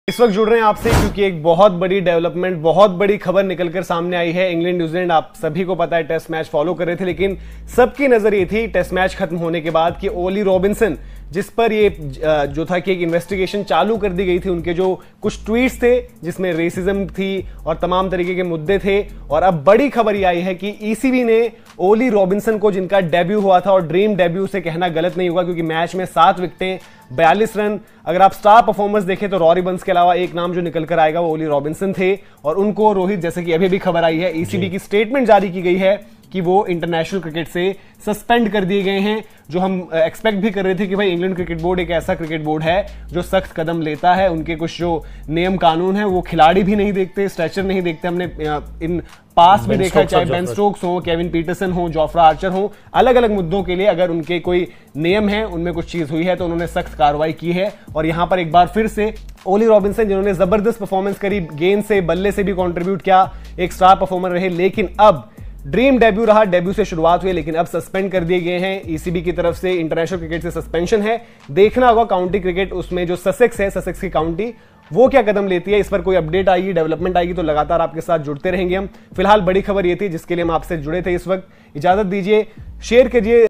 इंग्लैंड ने लगाया बड़े खिलाड़ी पर Ban, पहले ही मैच में लिए थे 7 विकेट
England big players banned from international cricket